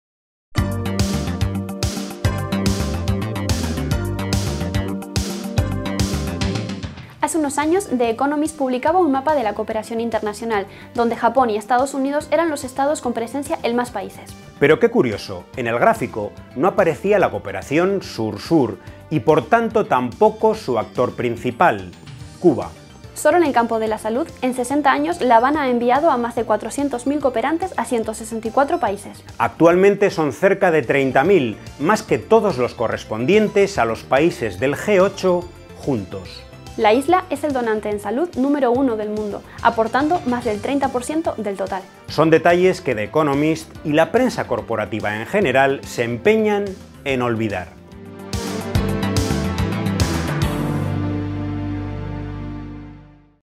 Cuba: ¿más cooperantes que todo el G8?
(+Italiano)

Cuba: più cooperanti dell'intero G8?

Hace unos años, The Economist publicaba un mapa de la cooperación internacional, donde Japón y EEUU eran los estados con presencia en más países.